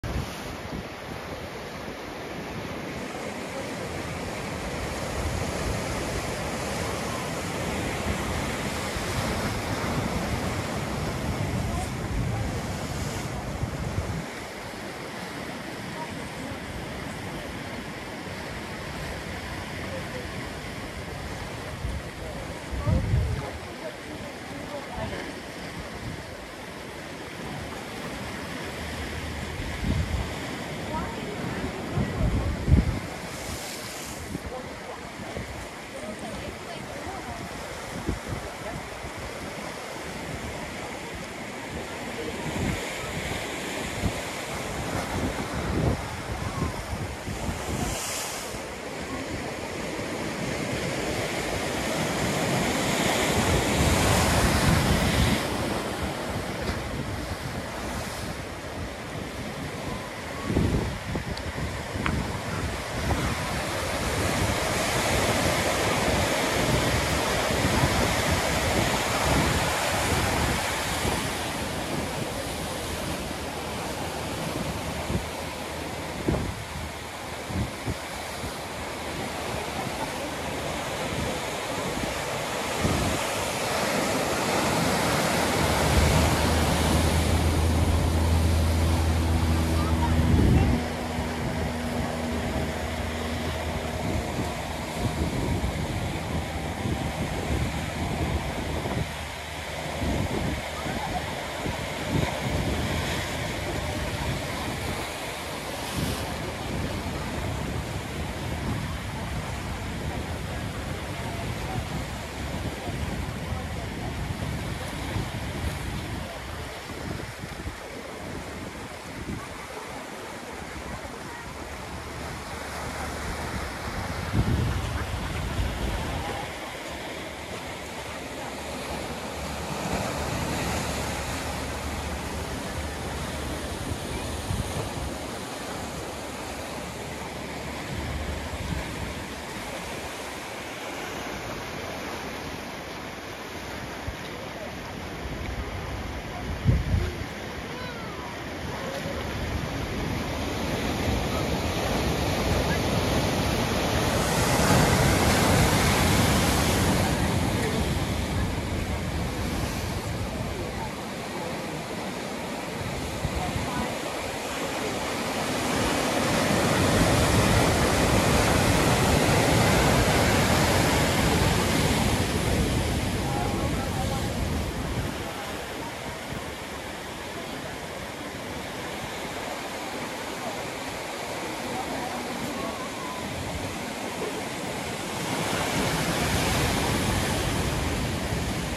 Laguna Beach: Thunderous White Foam + Orange Glow
The water's orange glow made me feel relaxed, energized, and in harmony with everything.

The white color foam and the thunder of crashing waves yanked me into the present moment.

The slight opening in the cloud right in the direction of the setting sun had an orange beam of light glowing the sky and the sea.

I lost myself in the moments—one with the sea, the sky, the earth, the air, and everything around me.